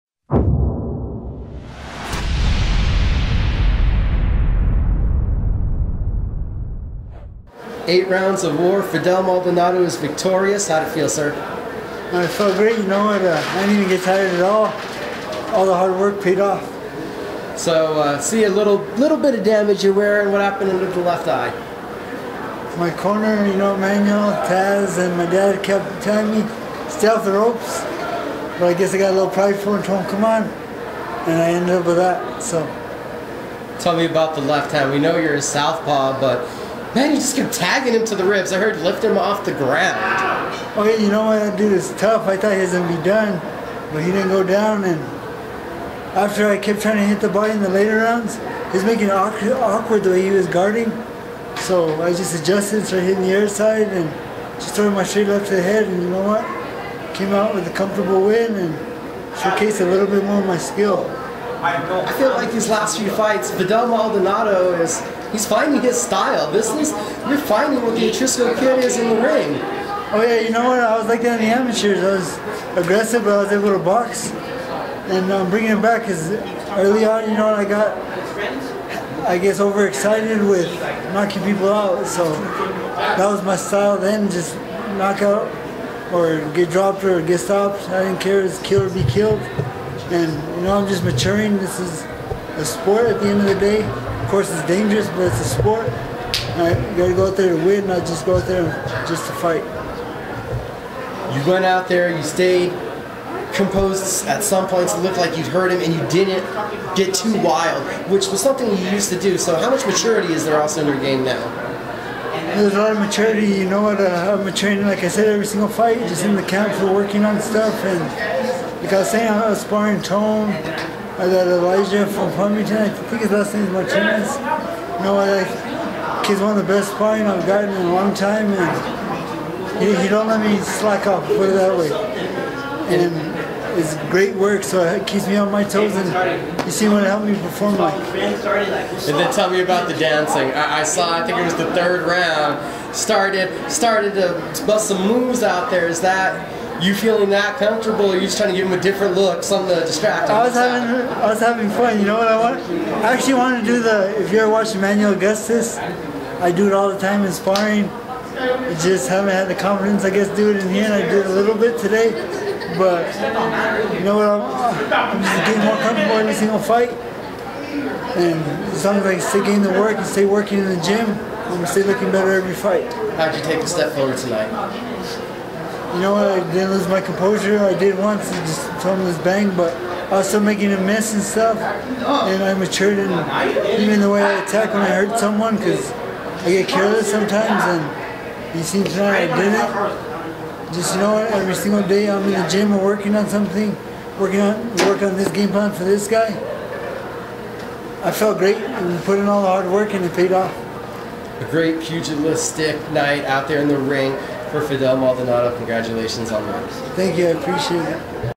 Fidel Maldonado Jr Fighting to Win Not Just Fight
The Atrisco Kid talks to Mika Frankl about his unanimous decision win over Johnny Rodriguez in the main event of Holmes Boxing's "Duke City Lions"